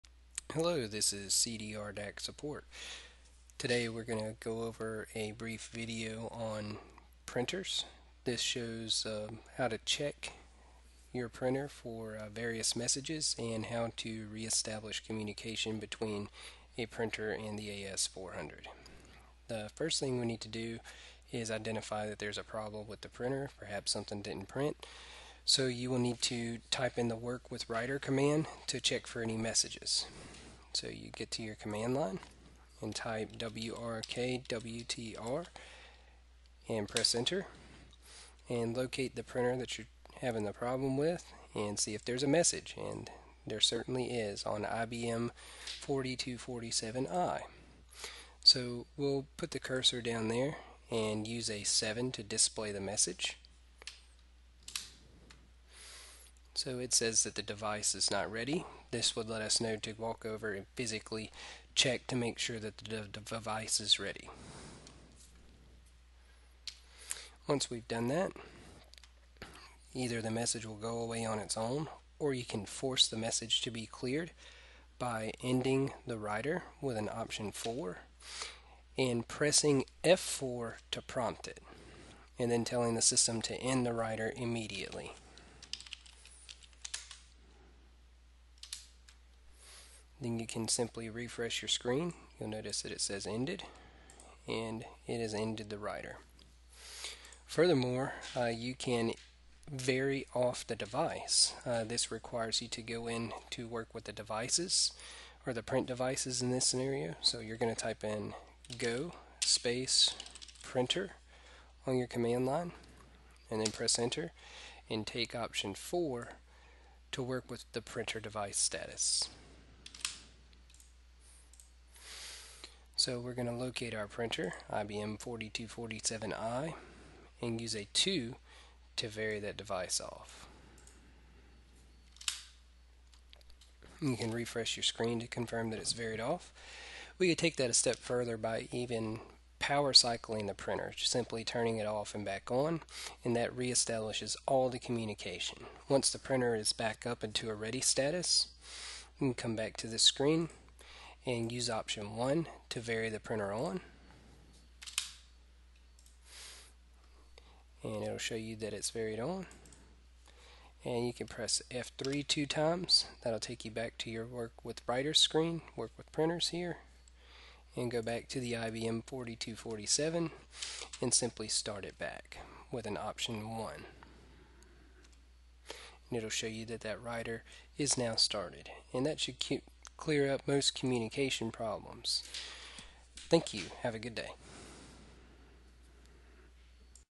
Restarting Print Writer
Restarting printer communication on the AS400.